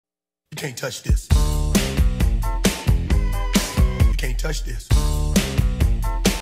FISHING FOR BIG BLUE CATFISH ON LAKE LIVINGSTON TEXAS
Lake Livingston blue catfish. Take a kid fishing, the memory lasts a lifetime. Floating jugs in 4 feet of water in the stump field. Had several break 50 pound leads. These fileted up nicely and tasted great. My grandson and myself making a memory for a lifetime.